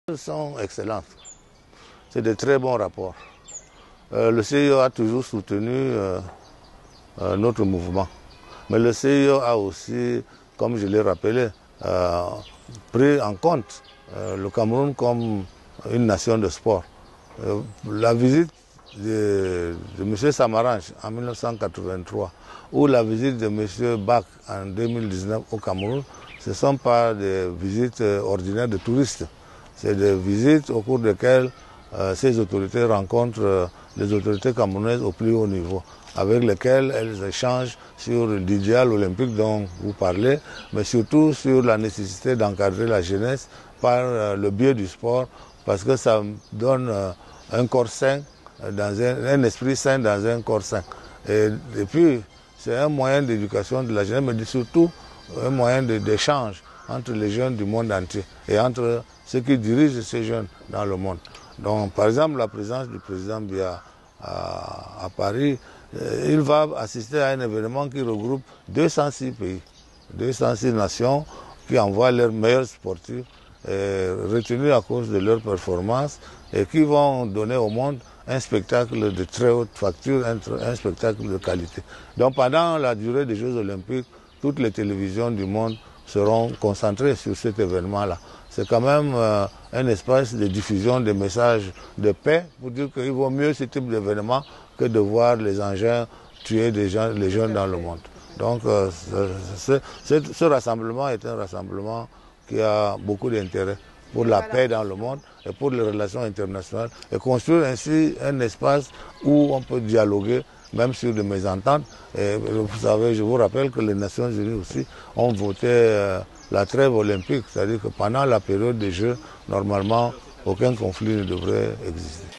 « Interview Président CNOSC avant les JO 2024 »
Entretien avec le President du Comité National Olympique et Sportif du Cameroun, Hamad Kalkaba Malboum, sur les relations entre le Cameroun et le Comité International Olympique.